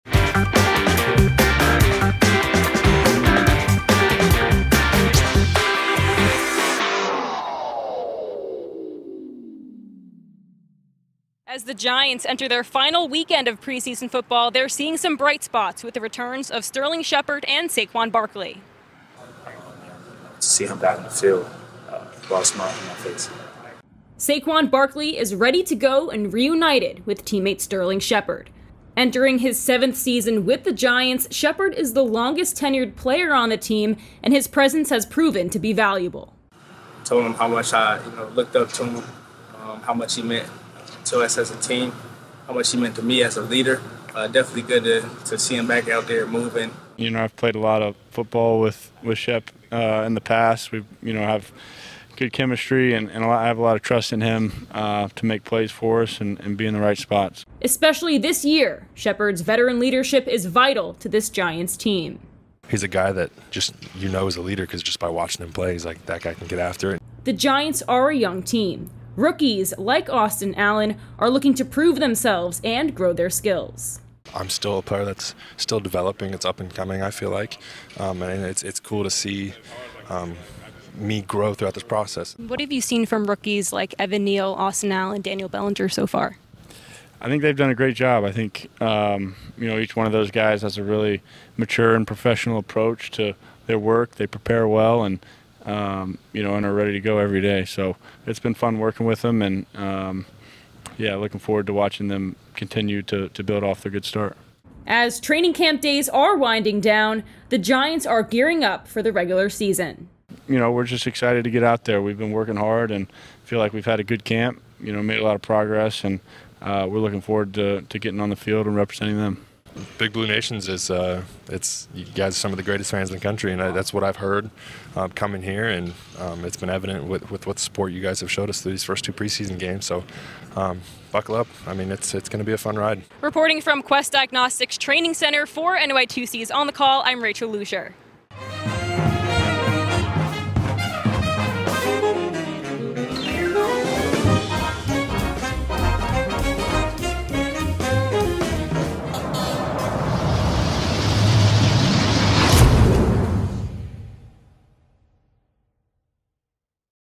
Giants WR Sterling Shepard Makes Highly Anticipated Return
Rachel Luscher looks at how the New York Giants offense is preparing for the 2022 season for NY2C's On The Call, executive produced by Carousso Enterprises.